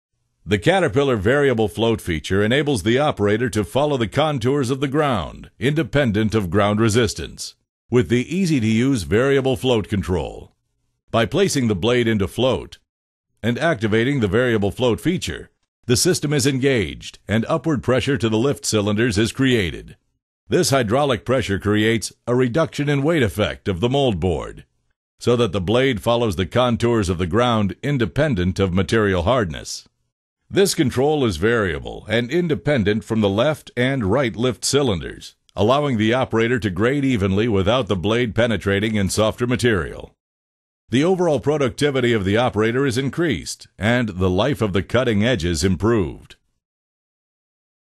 Variable Float Feature | Cat® M Series Motor Graders
Learn about the advantages of Variable Blade Float on Cat M Series Motor Graders.  for more information about our M Series Motor Graders.